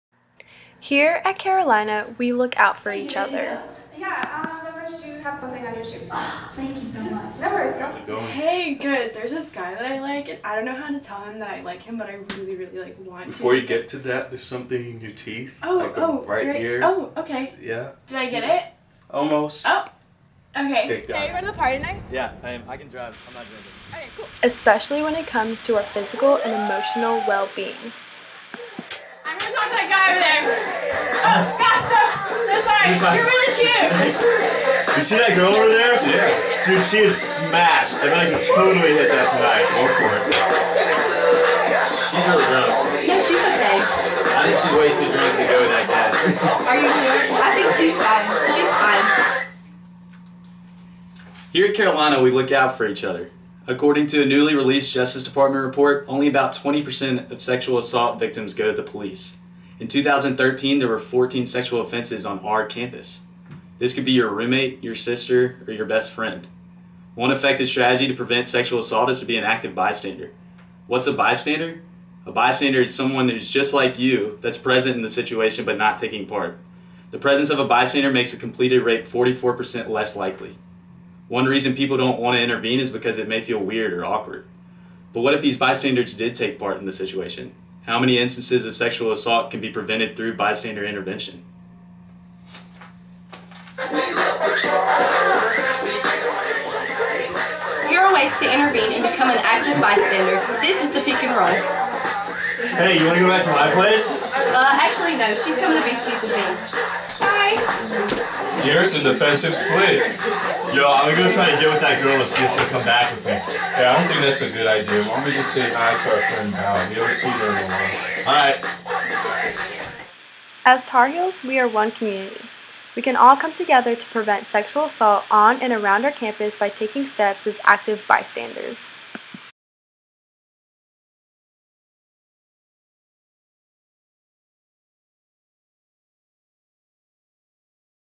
Sexual Assault Intervention: Be an active bystander
A team from Dr. Buzinski's Psyc566 class at the University of North Carolina made this video about sexual assault intervention. Please watch and then share it with 10 friends. Thanks!